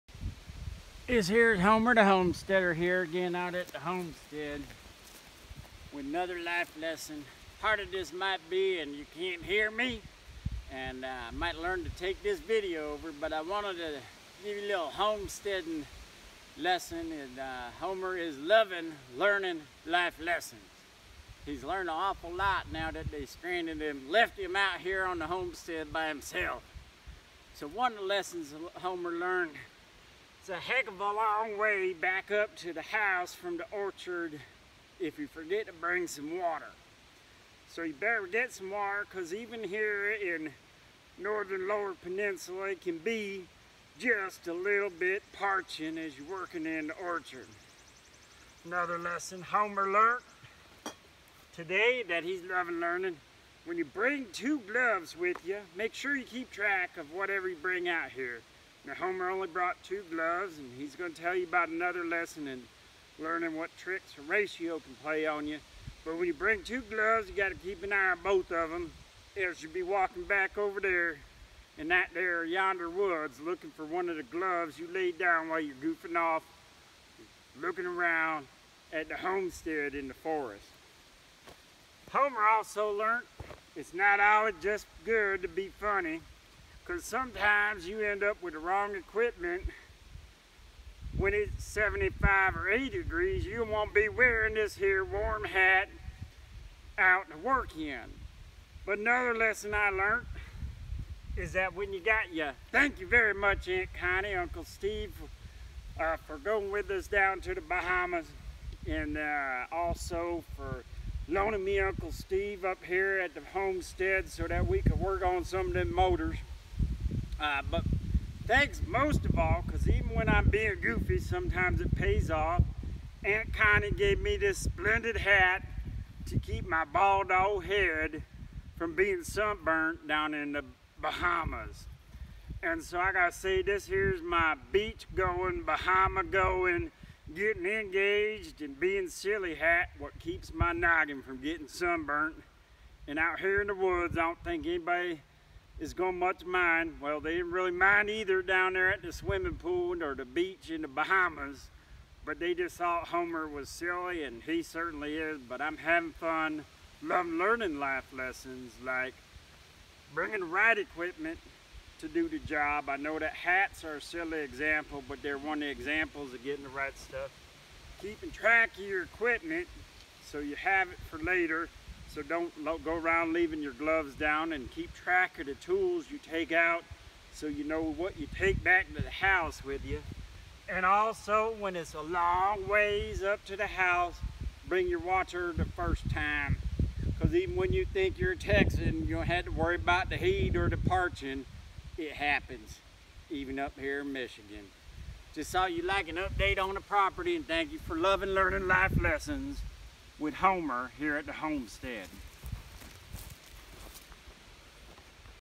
HILLLLL Episode 6: Working in the Orchard is a good place to learn!
Learning to keep track of equipment, use the right equipment and bring water along!